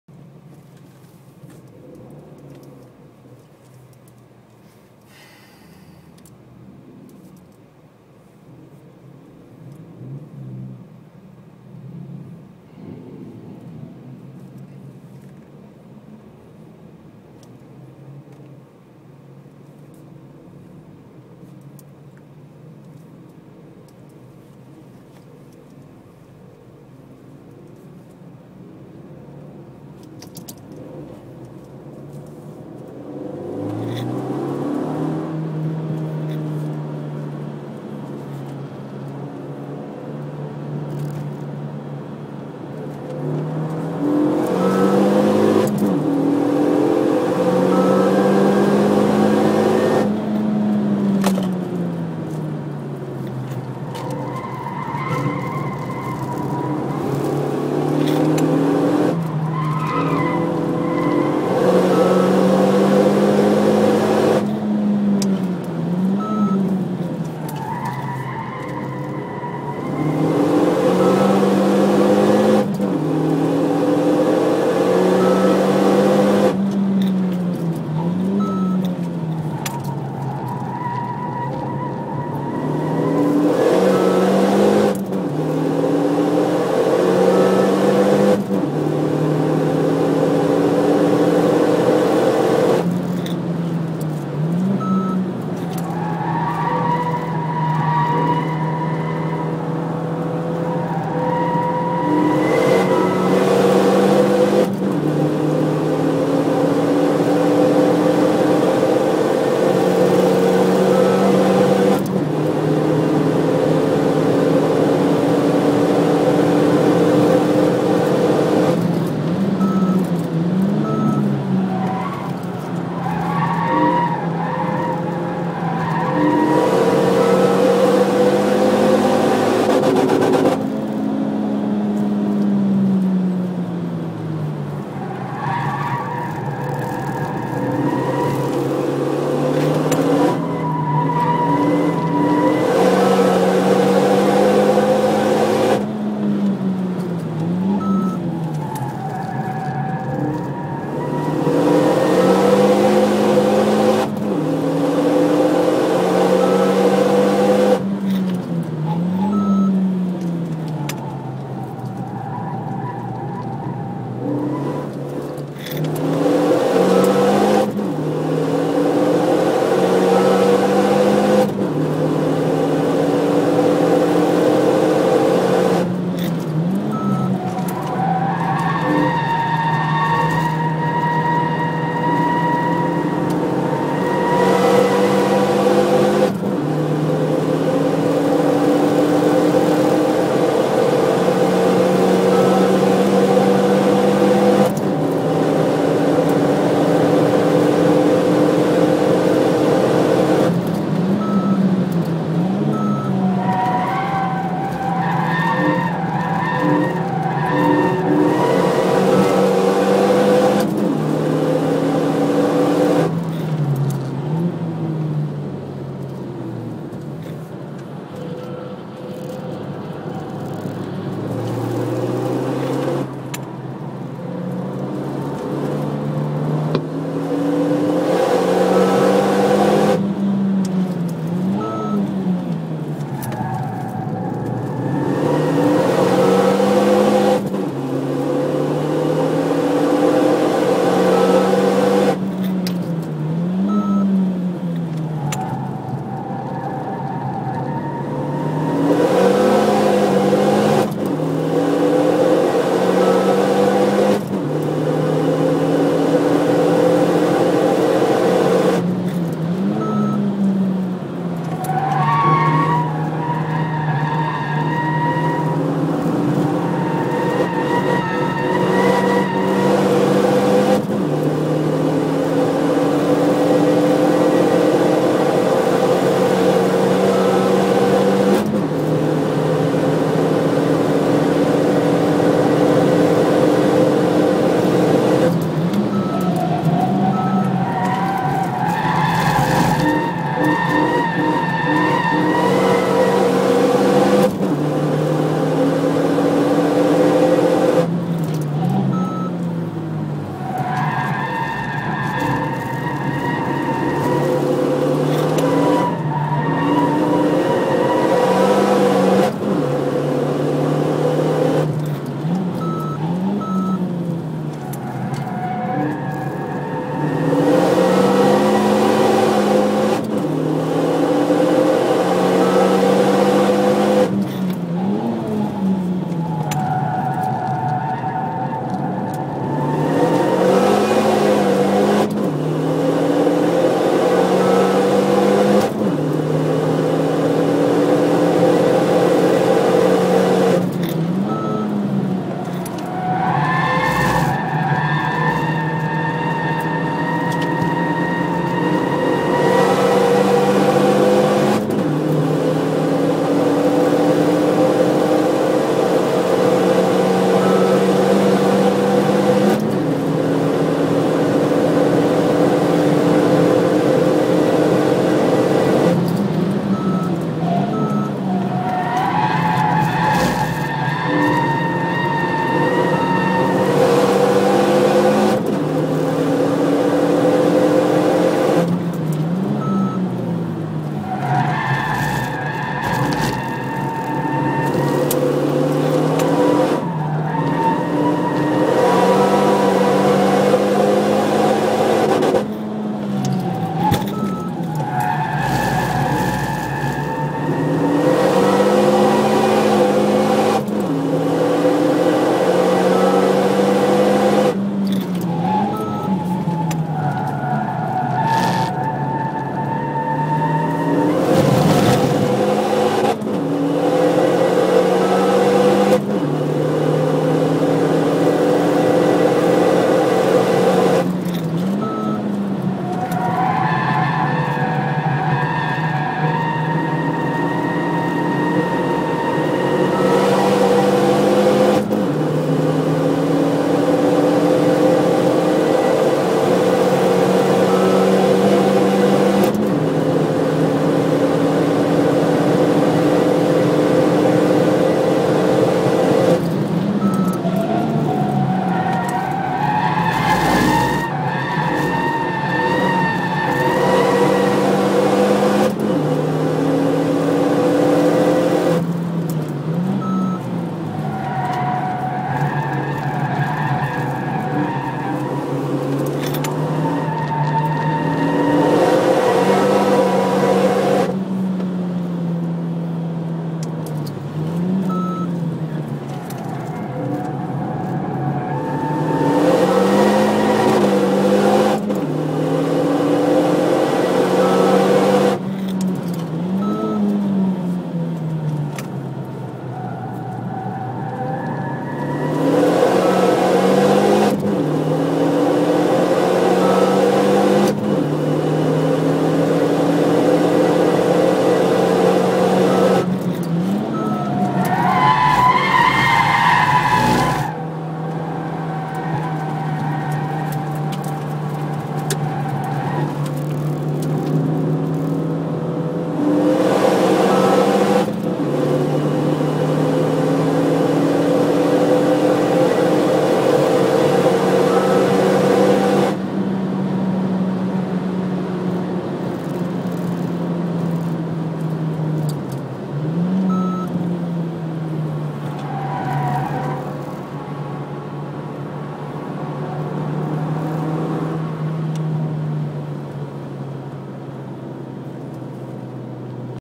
Hampton Downs, Megane RS250
Uncles twilight session, quick fang. Stock car.